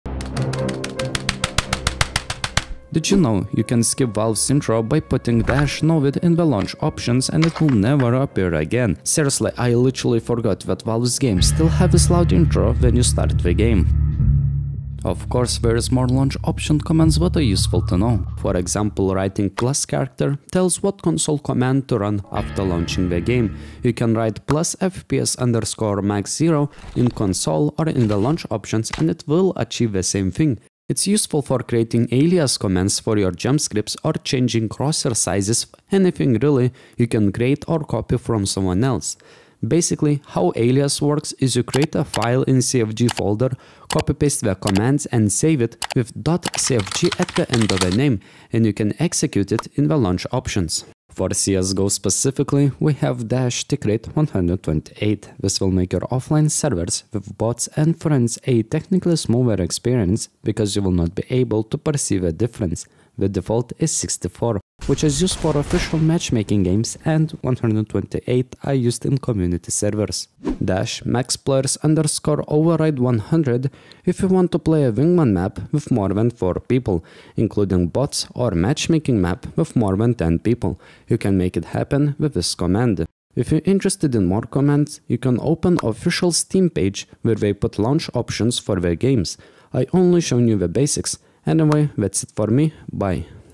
Steam Launch options
Commands used in video:
-novid
+fps_max 0
-tickrate 128/64
-maxplayers_override 100